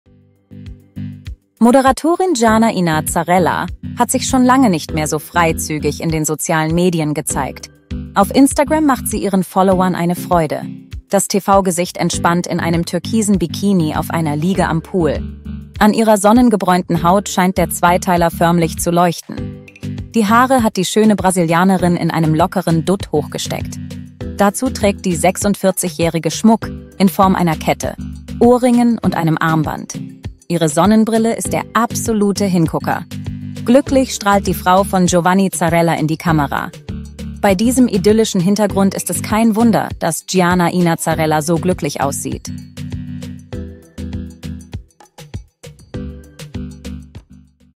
Jana Ina Zarrella zeigt sich traumhaft im Bikini • PROMIPOOL
Jana Ina Zarrella gehört zu den schönsten Moderatorinnen Deutschlands. Das beweist sie auch in den sozialen Medien. Auf Instagram teilt sie einen Bikini-Post.